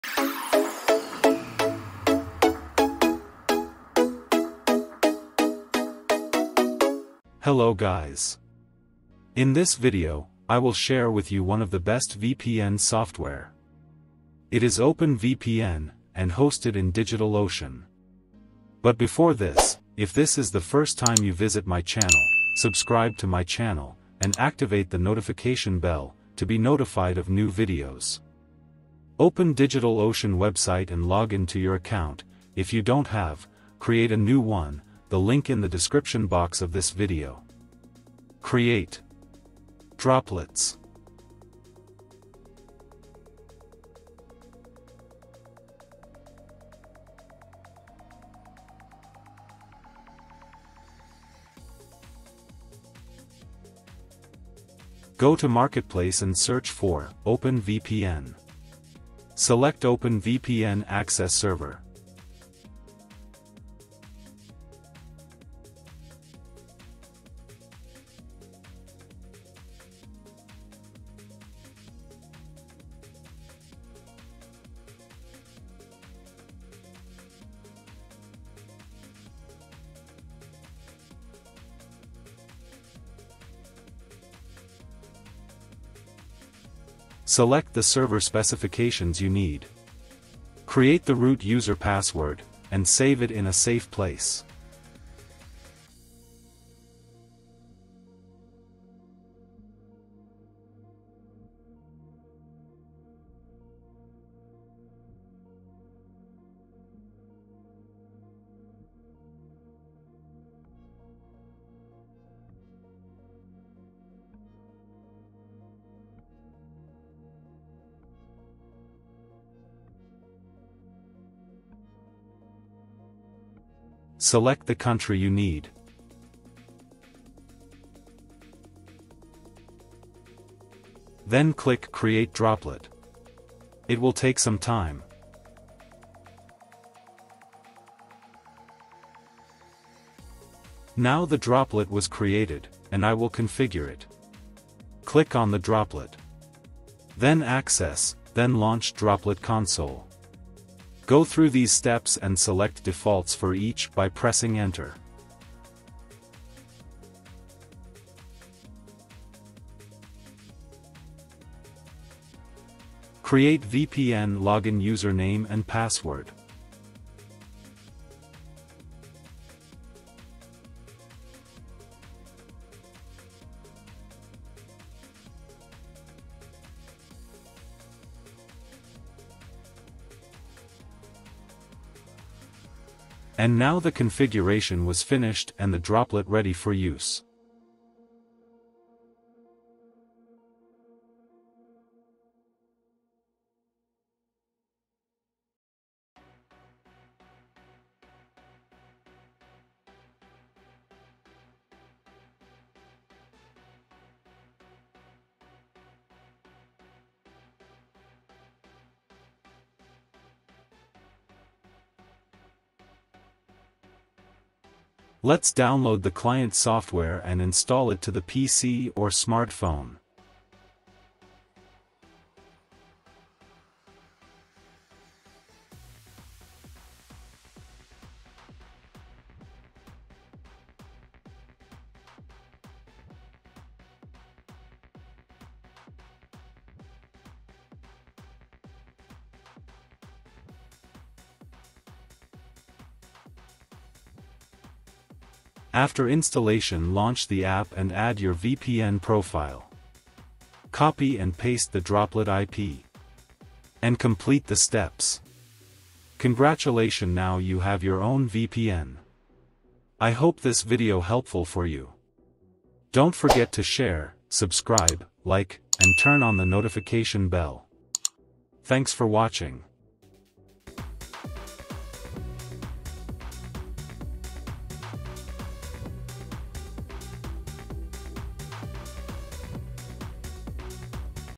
How to make your own vpn server Digital Ocean OpenVPN
Hello guys,
in this video, I will share with you one of the best VPN software,
It is Open VPN, and hosted in digital ocean

create
 droplets
go to marketplace and search for, open vpn

select open vpn access server
select the server specifications you need
create the root user password, and save it in safe place
select the country you need
then click create droplet
it will take some time
now the droplet was created, and I will configure it
click on the droplet
then access, then launch droplet console
go through these steps and select defaults for each by pressing enter
create vpn login user name and password
and now the configuration was finished and the droplet ready for use
lets download the client software and install it to the pc or smart android phone
after installation launch the app and add your vpn profile
copy and paste the droplet IP
and complete the steps
congratulation now you have your own VPN

00:00 intro
00:30 login to the digital ocean website and create a droplet
2:25 Configure OpenVPN access server
3:36 Download and install the OpenVPN client
4:05 Connect to VPN